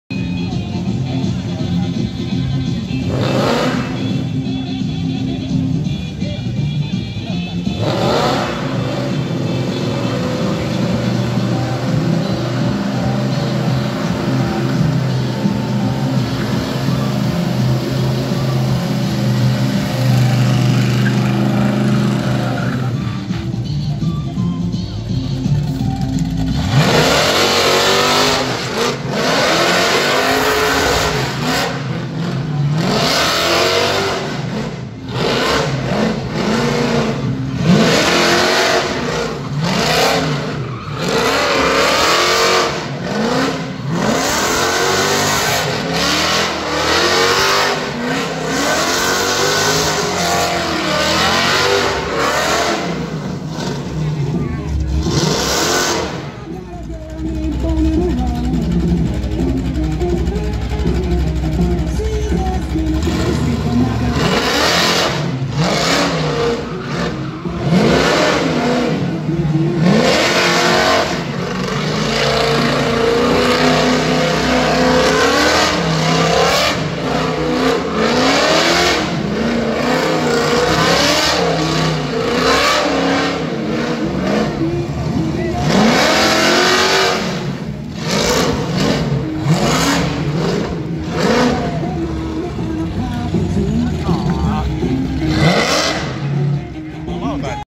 Dodge Coronet burnout, Koria 1.10.22
End of season cruising & burnout pary by Concrete garage II